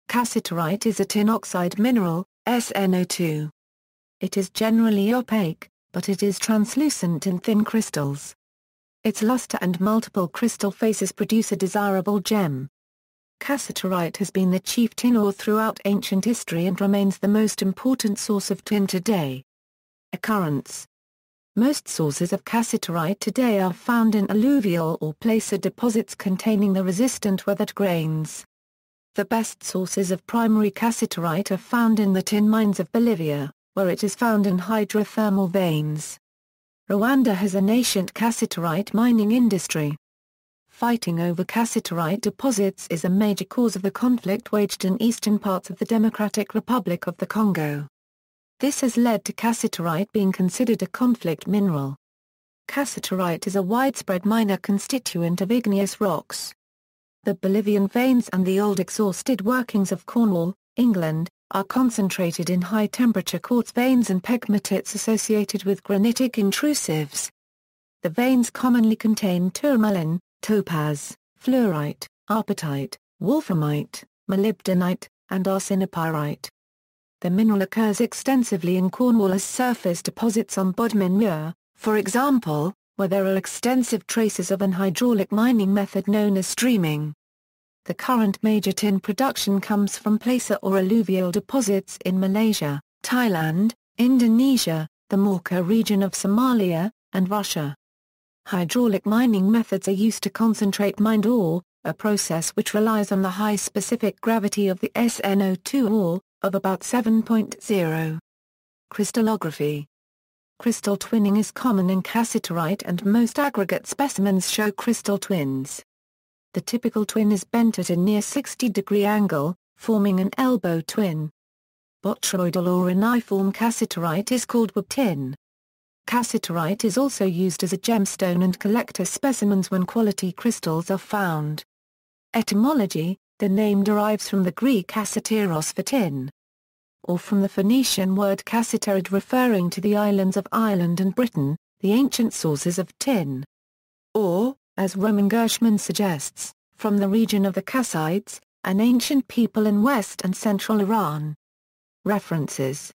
Cassiterite
Cassiterite is a tin oxide mineral, SnO2. It is generally opaque, but it is translucent in thin crystals. Its luster and multiple crystal faces produce a desirable gem. Cassiterite has been the chief tin ore throughout ancient history and remains the most important source of tin today.